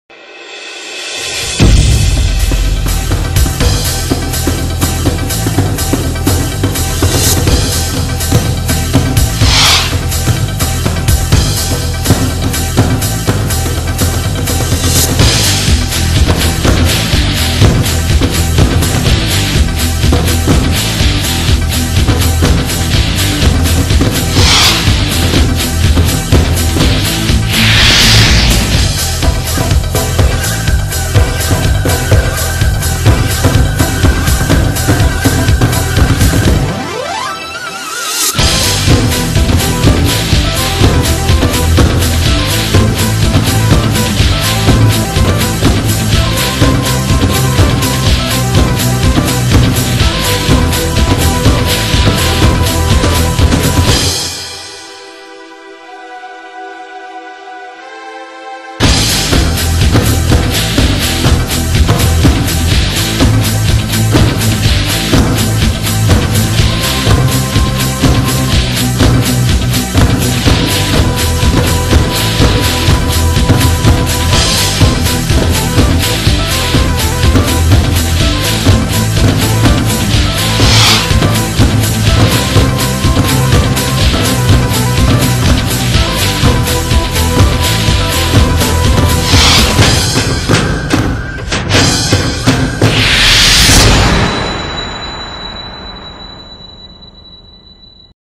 japan 01 43(Drum CaT Japanese Version)
DrumCat Performance, Marvelous sounds of drums. Japanese Version.

드럼캣 퍼포먼스. 드럼이 내는 신비로운 소리들. 일본어 버전